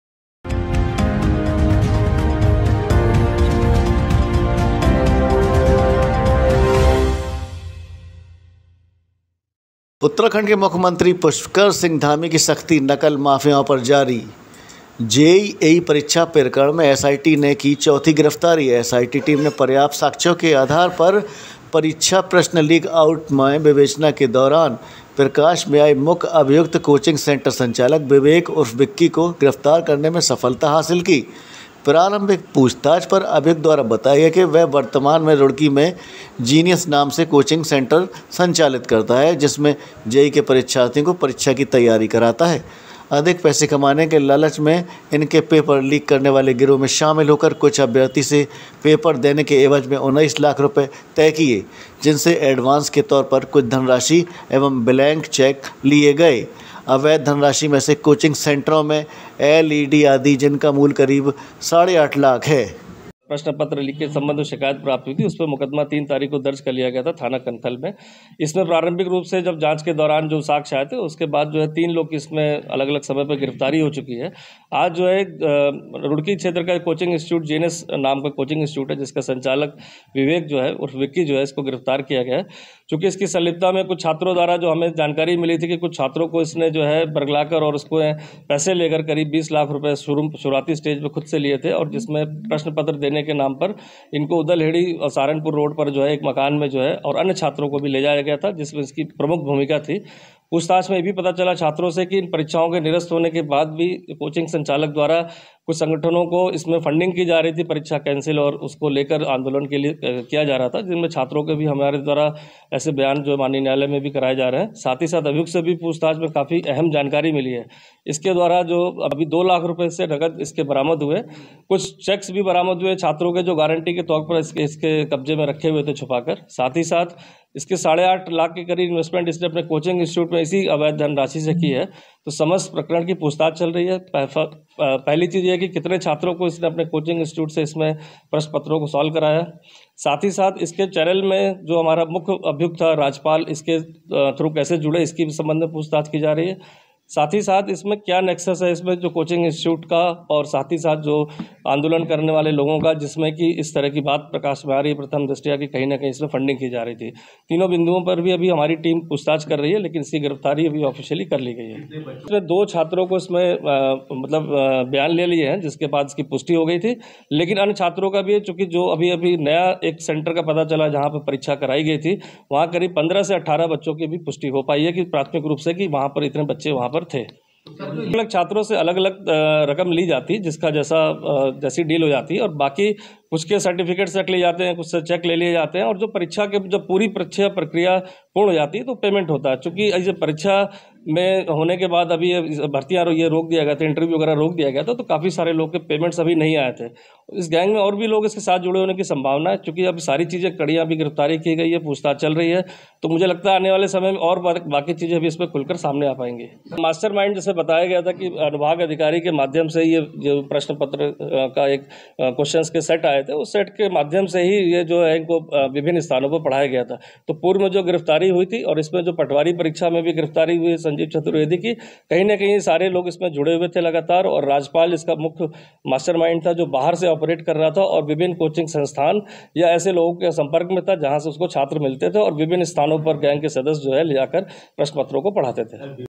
हरिद्वार : J.E./A.E. प्रश्न लीक प्रकरण में S.I.T. हर एंगल से कर रही है जांच, हर आरोपी का जेल जाना तय
मुख्यमंत्री श्री पुष्कर सिंह धामी की सख्ती नकल माफियाओं पर जारी, जेई/एई परीक्षा प्रकरण में S.I.T. ने की चौथी गिरफ्तारी S.I.T. टीम ने पर्याप्त साक्ष्यों के आधार पर उक्त परीक्षा प्रश्न लीक आउट करने में विवेचना के दौरान प्रकाश में आए मुख्य अभियुक्त कोचिंग सेंटर संचालक विवेक ऊर्फ विक्की को गिरफ्तार करने में सफलता प्राप्त की गई है ।